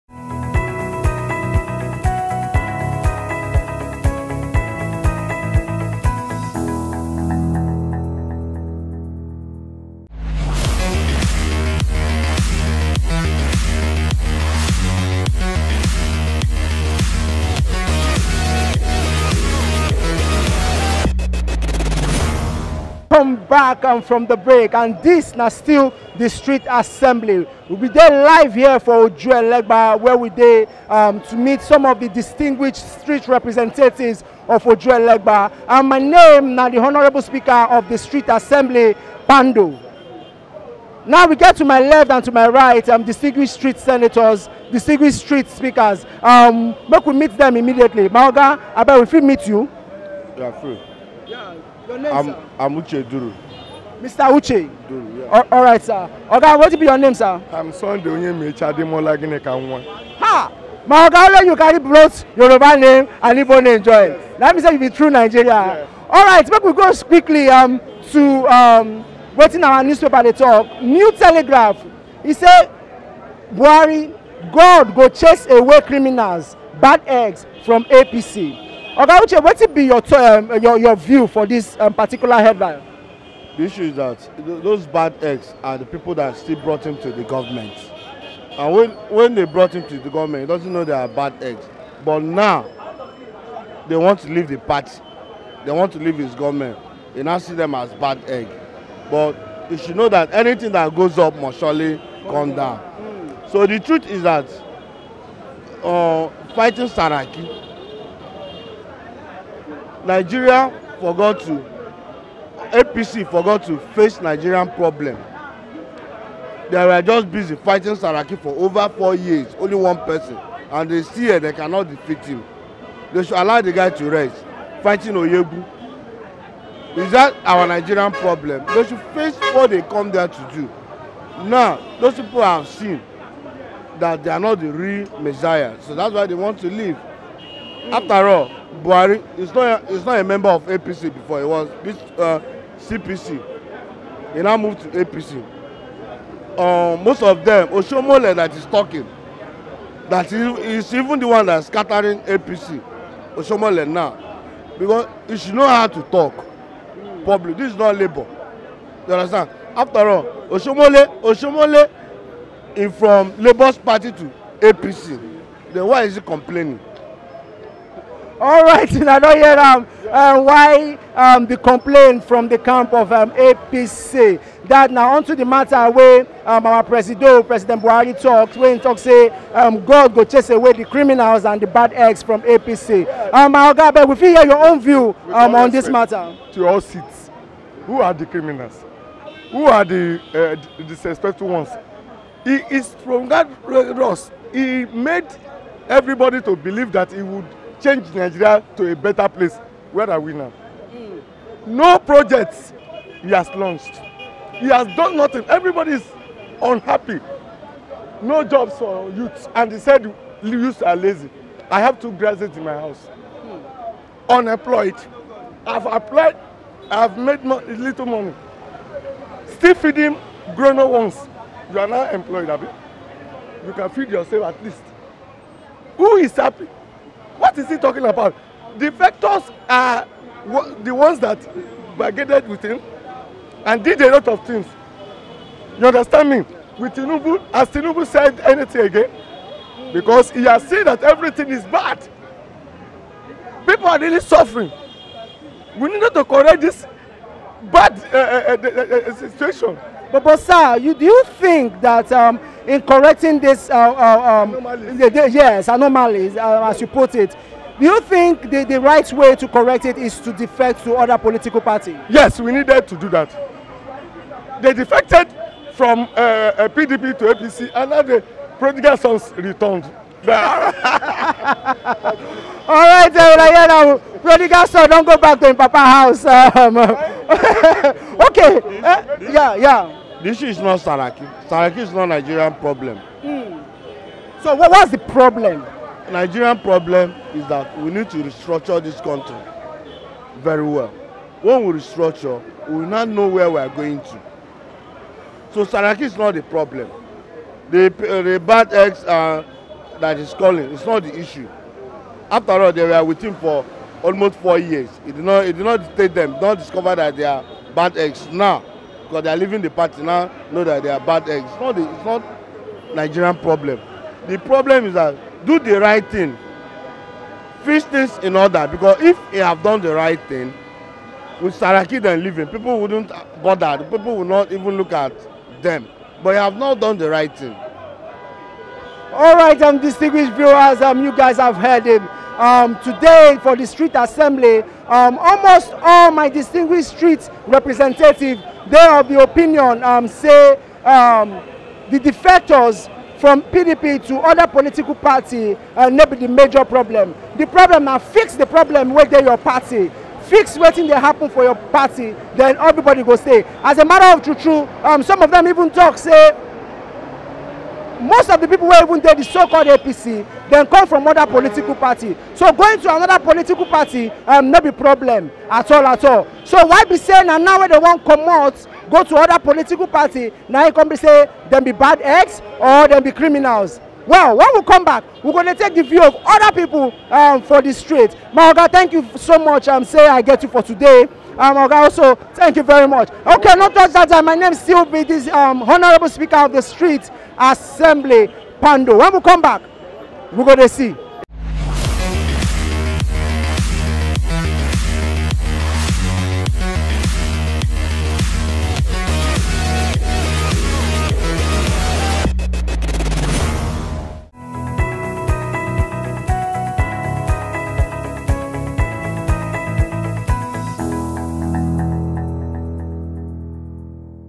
"OSHIOMHOLE DOES NOT UNDERSTAND THE DIFFERENCE BETWEEN POLITICS AND LABOUR"
SEE WHAT NIGERIANS ARE SAYING ABOUT BUHARI'S COMMENTS ABOUT POLITICIANS DEFECTING FROM APC